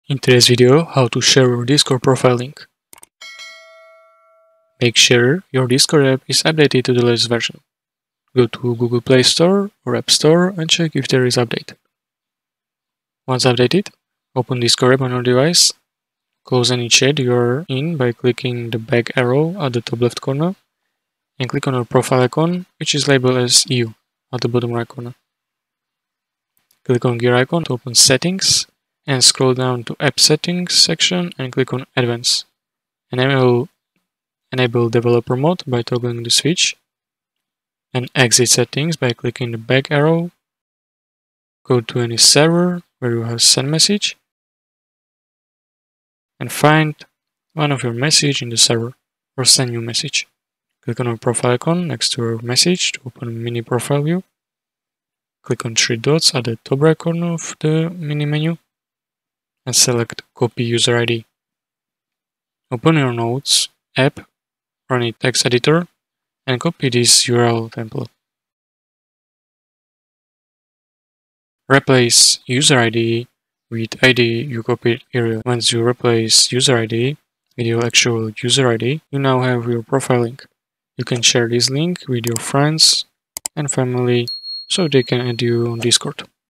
How To Share Your Profile Link On Discord | Copy Discord Profile Link (2025)
Thank you very much for your support!

►iPhone/Android?

► SAMSUNG Odyssey G50A

I'm going to show you how to share your Discord profile link with others. If you want to share the link to your Discord profile, follow these steps. Let's get started!

### Steps to Share Your Discord Profile Link

1. **Update Discord**:
   - Make sure your Discord app is updated to the latest version.

2. **Open Discord**:
   - Open the Discord app on your device.

3. **Navigate to Your Profile**:
   - Close any chat you are in by clicking the back arrow at the top left corner.
   - Click on your profile icon, which is labeled as "You" at the bottom right corner.

4. **Open Settings**:
   - Click on the gear icon to open your settings.

5. **Enable Developer Mode**:
   - Scroll down to the "App Settings" section and click on "Advanced."
   - Enable "Developer Mode" by toggling the switch.

6. **Copy Your User ID**:
   - Exit the settings by clicking the back arrow at the top left corner.
   - Go to any server where you have sent a message.
   - Find one of your messages in the server, or send a new message.
   - Click on your profile icon next to your message to open the mini profile view.
   - Click on the three dots at the top right corner of this mini profile view.
   - Select "Copy User ID."

7. **Create Your Profile Link**:
   - Open your notes app or any text editor.
   - Replace `UserID` with the ID you copied earlier.

8. **Share Your Profile Link**:
   - Once you have replaced `UserID` with your actual user ID, you now have your profile link.
   - You can share this link with your friends and family so they can add you on Discord.

### Example

If your user ID is `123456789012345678`, your profile link would look like this:

By following these simple steps, you can easily share your Discord profile link. If you have any questions, drop them in the comments below.